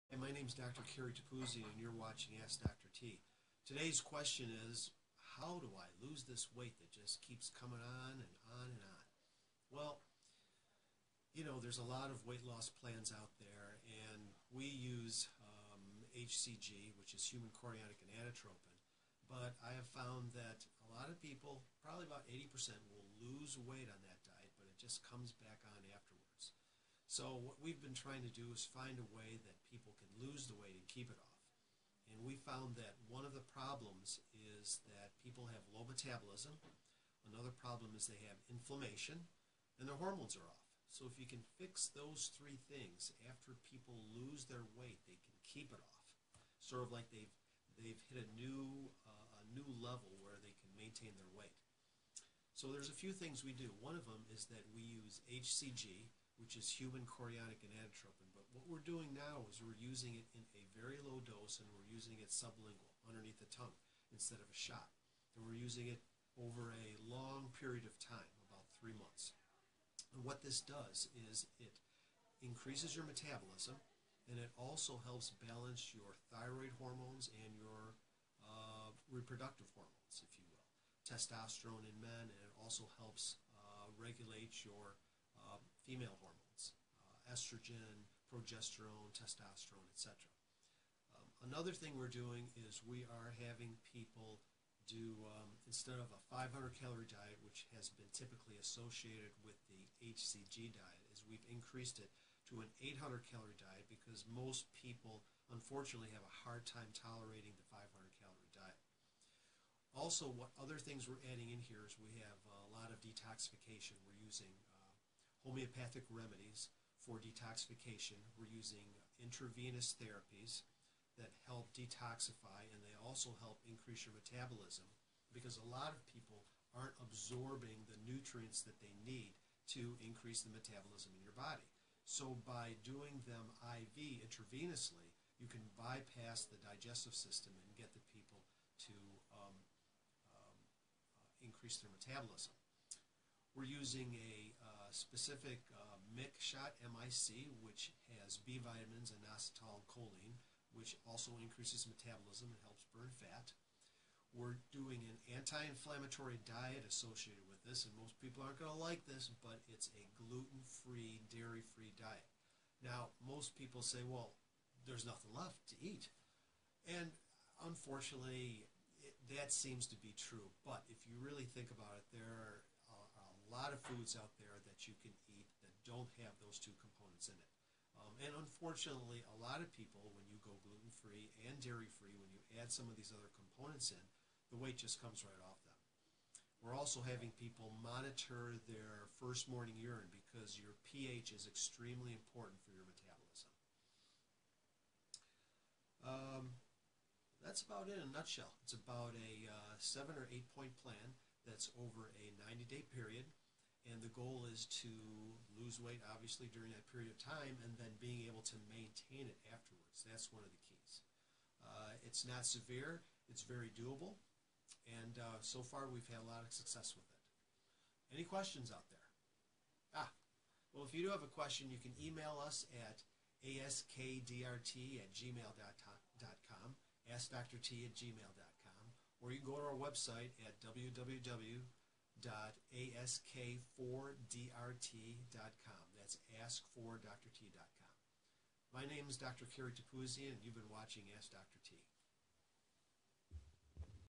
Modified HCG Diet Plan
DR.T discusses his modified HCG weight loss plan